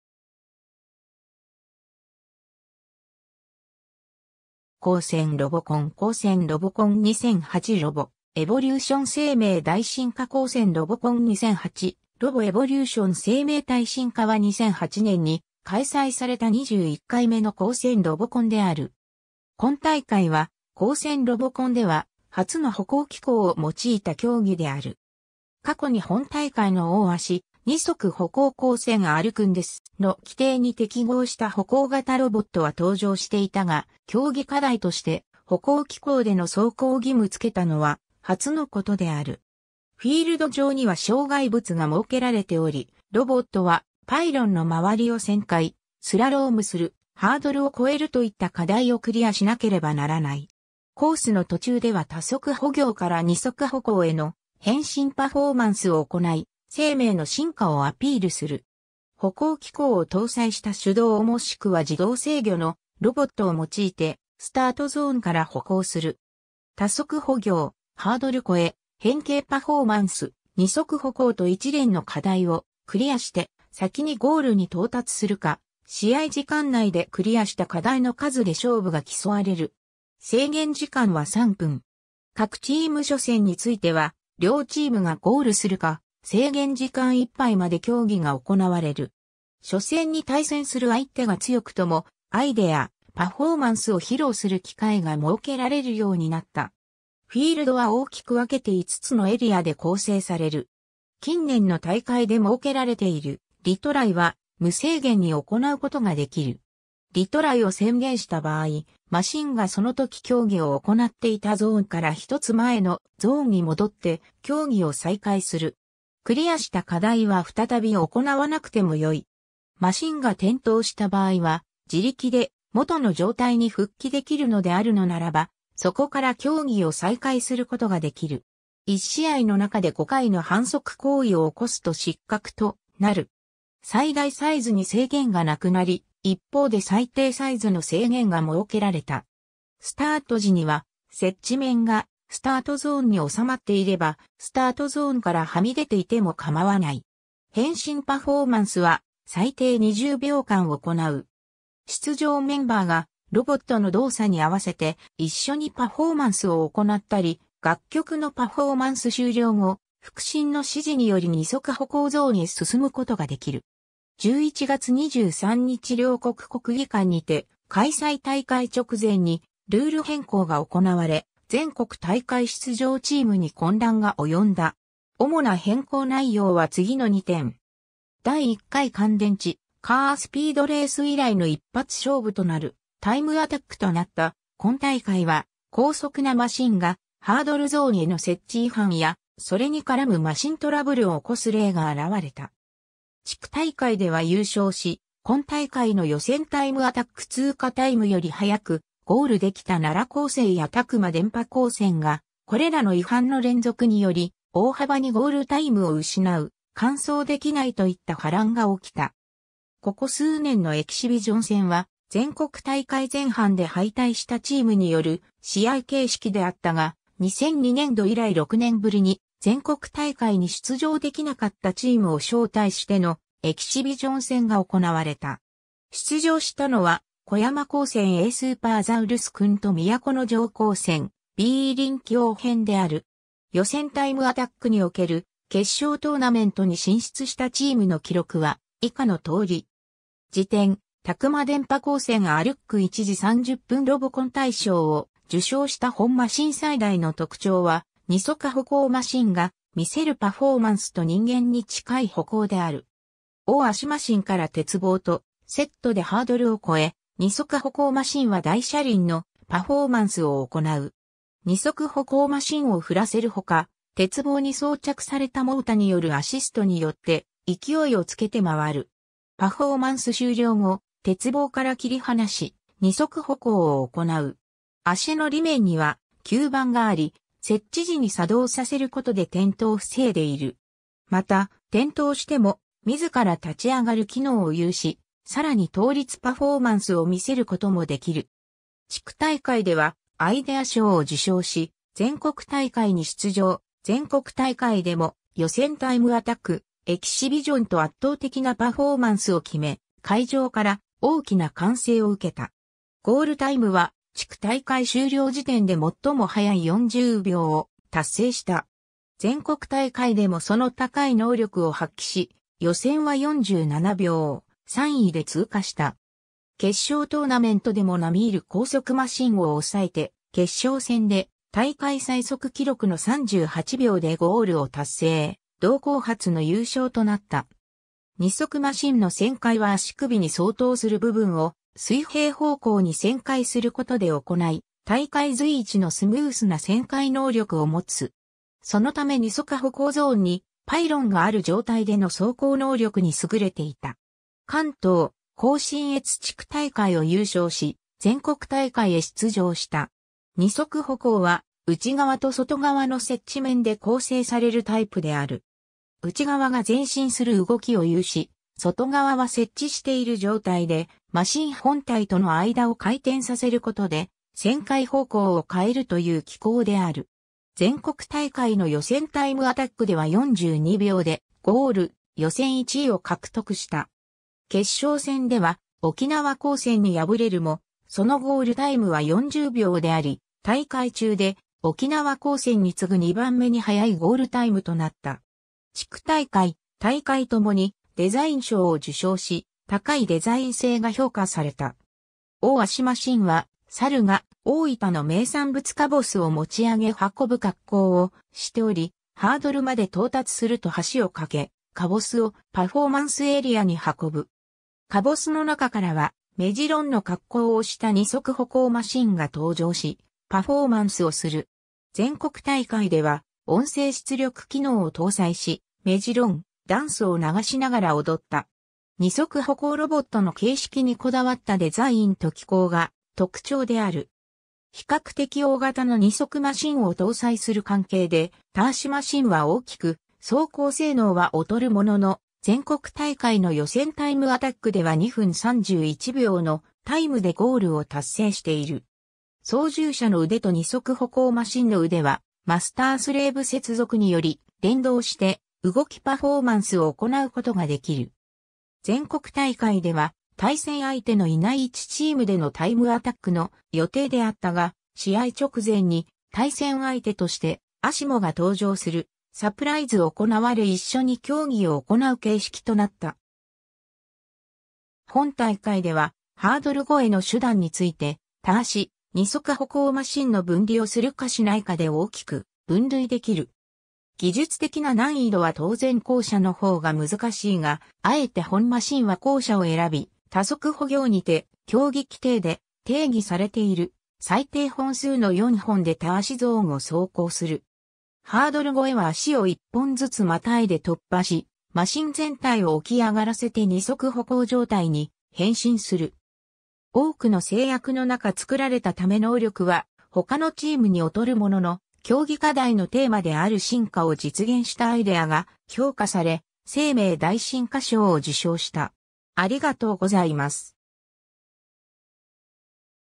高専ロボコン2008 ROBO-EVOLUTION 生命大進化
高専ロボコン 」 高専ロボコン2008 ROBO-EVOLUTION 生命大進化 高専ロボコン2008 ROBO-EVOLUTION 生命大進化（こうせんロボコン2008 ロボエボリューション せいめいだいしんか）は2008年に開催された21回目の高専ロボコンである。
 今大会は高専ロボコンでは初の歩行機構を用いた競技である。
過去に本大会の多足（福島高専 たこのデビット君 等）、2足歩行（育英（現サレジオ）高専 歩くんです）の規定に適合した歩行型ロボットは登場していたが、競技課題として歩行機構での走行義務付けたのは初のことである。
 フィールド上には障害物が設けられており、ロボットはパイロンの周りを旋回、スラロームする、ハードルを越えるといった課題をクリアしなければならない。
コースの途中では多足歩行から2足歩行への変身パフォーマンスを行い、生命の進化をアピールする。
 歩行機構を搭載した手動もしくは自動制御のロボットを用いて、スタートゾーンから歩行する。
多足歩行、ハードル越え、変形パフォーマンス、2足歩行と一連の課題をクリアして、先にゴールに到達するか、試合時間内でクリアした課題の数（走破した距離ではない）で勝負が競われる。
制限時間は3分。
各チーム初戦については、両チームがゴールするか制限時間いっぱいまで競技が行われる。
初戦に対戦する相手が強くとも、アイデア、パフォーマンスを披露する機会が設けられるようになった。
 フィールドは大きく分けて5つのエリアで構成される。
 近年の大会で設けられているリトライは、無制限に行うことができる。
リトライを宣言した場合、マシンがその時競技を行っていたゾーンから一つ前のゾーンに戻って競技を再開する。
クリアした課題は再び行わなくてもよい。
マシンが転倒した場合は、自力で元の状態に復帰できるのであるのならば、そこから競技を再開することができる。
（転倒して進んだ場合は、再開位置が指示される。）1試合の中で5回の反則行為を起こすと失格となる。
 最大サイズに制限がなくなり（安全上の面から制限される場合がある）、一方で最低サイズの制限が設けられた。
スタート時には接地面がスタートゾーンに収まっていれば、スタートゾーンからはみ出ていても構わない。
 変身パフォーマンスは最低20秒間行う。
出場メンバーがロボットの動作に合わせて一緒にパフォーマンスを行ったり、楽曲のパフォーマンス終了後、副審の指示により2足歩行ゾーンへ進むことができる。
 11月23日 両国国技館にて開催 大会直前にルール変更が行われ、全国大会出場チームに混乱が及んだ。
主な変更内容は次の2点。
 第1回 乾電池カースピードレース以来の一発勝負となるタイムアタックとなった今大会は、高速なマシンがハードルゾーンへの接地違反やそれに絡むマシントラブルを起こす例が現れた。
地区大会では優勝し今大会の予選タイムアタック通過タイムより速くゴールできた奈良高専や詫間電波高専が、これらの違反の連続により大幅にゴールタイムを失う、完走できないといった波乱が起きた。
 ここ数年のエキシビジョン戦は全国大会前半で敗退したチームによる試合形式であったが、2002年度以来6年ぶりに全国大会に出場できなかったチームを招待してのエキシビジョン戦が行われた。
出場したのは小山高専A SUPERザウルス君と都城高専B 麟麒応変である。
 予選タイムアタックにおける、決勝トーナメントに進出したチームの記録は以下のとおり。
 次点: 詫間電波高専 ALK 1:30 ロボコン大賞を受賞した本マシン最大の特徴は、2足歩行マシンが見せるパフォーマンスと人間に近い歩行である。
 多足マシンから鉄棒とセットでハードルを越え、2足歩行マシンは大車輪のパフォーマンスを行う。
2足歩行マシンを振らせるほか、鉄棒に装着されたモータによるアシストによって勢いをつけて回る。
パフォーマンス終了後、鉄棒から切り離し、2足歩行を行う。
足の裏面には吸盤があり、設置時に作動させることで転倒を防いでいる。
また、転倒しても自ら立ち上がる機能を有し、更に倒立パフォーマンスを見せることもできる。
 地区大会ではアイデア賞を受賞し全国大会に出場、全国大会でも予選タイムアタック、エキシビジョンと圧倒的なパフォーマンスを決め、会場から大きな歓声を受けた。
 ゴールタイムは地区大会終了時点で最も速い40秒を達成した。...